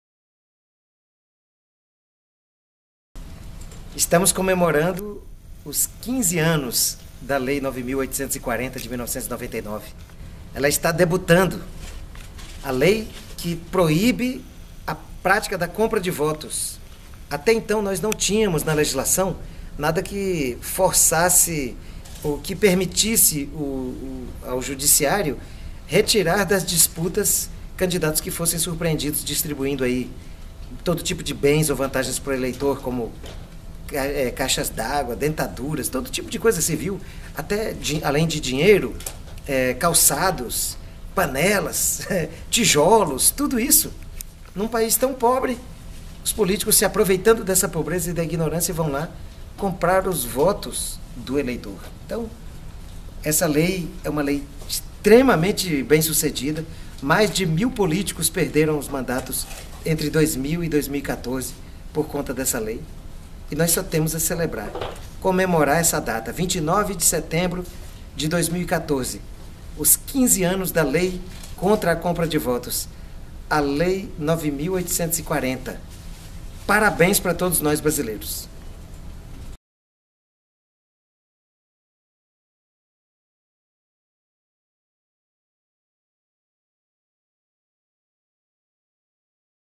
MCCE - Eleições 2014 - 15 anos da Lei Nº 9840/99 (Lei Contra a Compra de Votos)
O Movimento de Combate à Corrupção Eleitoral - MCCE
comemora os 15 anos de uma das maiores conquistas da sociedade brasileira, a Lei Nº 9840 de 1999, popularmente conhecida como "Lei Contra a Compra de Votos" ou "Lei dos Bispos". A primeira lei de iniciativa popular do Brasil.

De uma ideia surgida na CNBB, teve a adesão de dezenas de entidades nacionais, inclusive a OAB, que iniciaram a coleta de cerca de um milhão de assinaturas em todo o país.

Esta lei criminalizou a compra de votos permitindo que o judiciário retirasse das disputas eleitorais os candidatos que fossem surpreendidos distribuindo bens ou vantagens, inclusive dinheiro em troca de votos.

O povo brasileiro tem muito o que comemorar neste dia 29 de setembro de 2014.

15 anos da Lei Contra a Compra de Votos.
Parabéns Brasil !